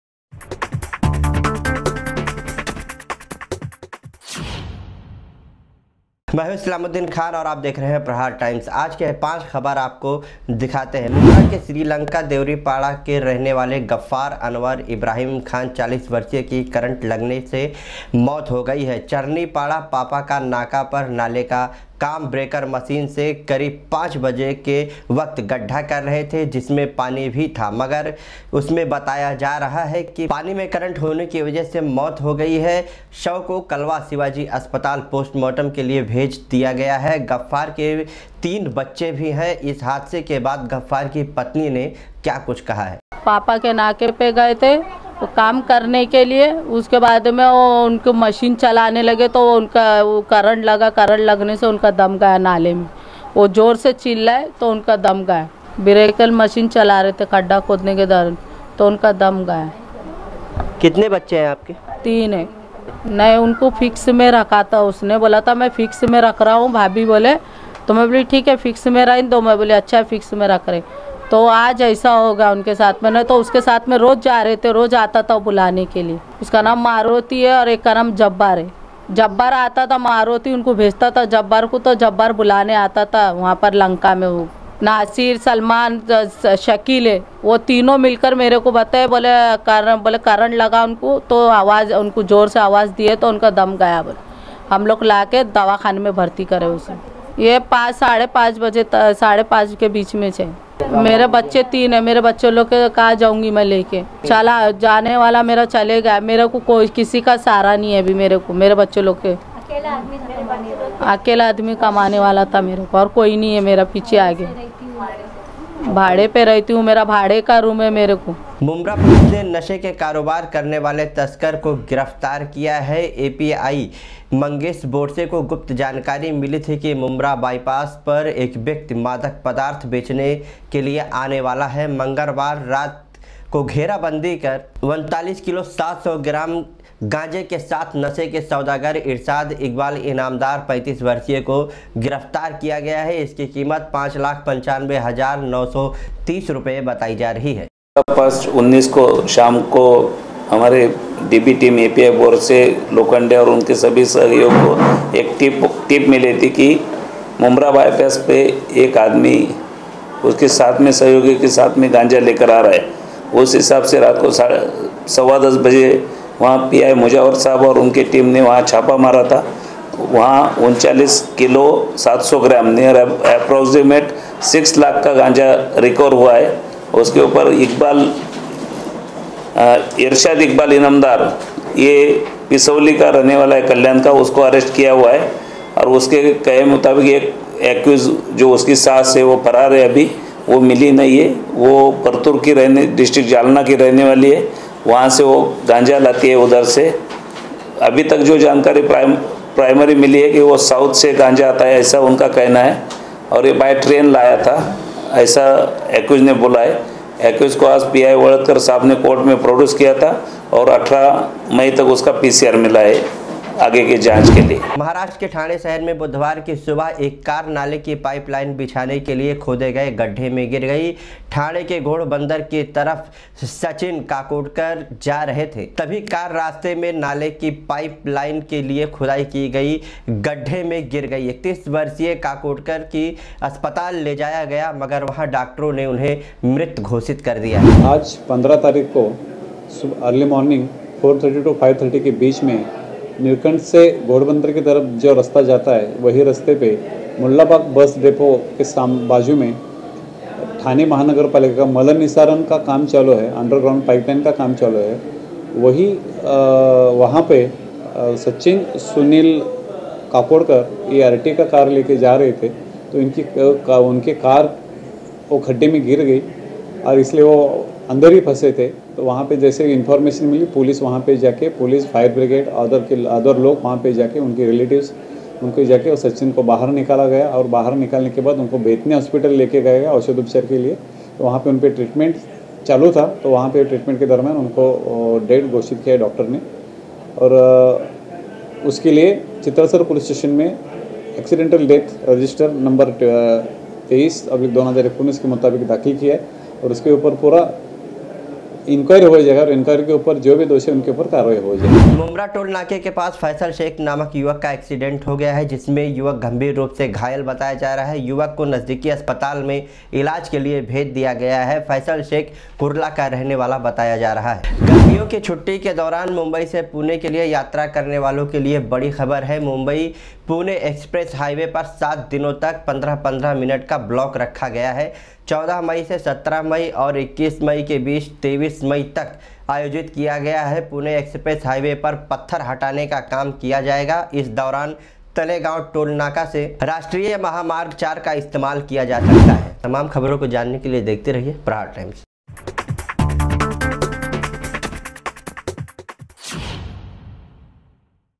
TOP 5 करंट लगने से हुई मौत,मुम्ब्रा में मादक पदार्थ के साथ युवक गिरफ्तार| Prahar Times
} काम के दौरान गफ्फार को लगा करंट हुई मौत

2}मुम्ब्रा पुलिस ने मादक पदार्थ के साथ आरोपी गिरफ्तार

3}पाइप लाइन की खुदी गड्ढे में गिरी कर,एक ब्यक्ति की मौत

4}मुम्ब्रा टोल नाके के पास फैसल नामक युवक का हुआ एक्सीडेंट

5}मुम्बई पुणे एक्सप्रेस रहेगा 15+15 मिनट ब्लॉक

prahar times